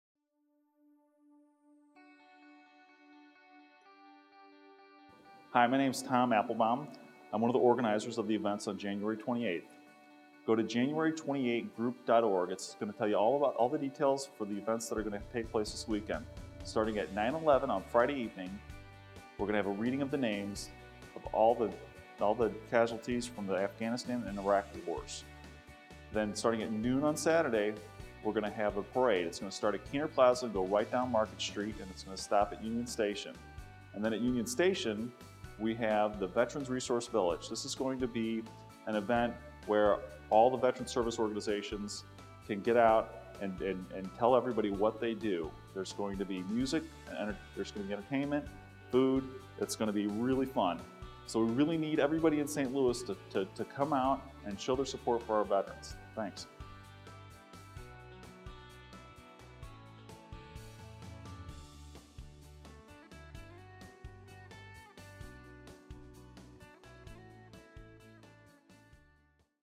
Tom Appelbaum - Organizer of January28Group.org
Tom Appelbaum, organizer of the encourages everyone to come out to support the end of the Iraq War by coming to on Friday at 9:11pm Reading of the Fallen (12 hours Candlelight Vigil).  Then on Saturday at noon, you should come to the Welcome Home the Troops Parade.  After the Parade, there will be a Veterans Resource Village at Union Station.  Live Music, food and lots of opportunities for Vets.  Bring the entire family!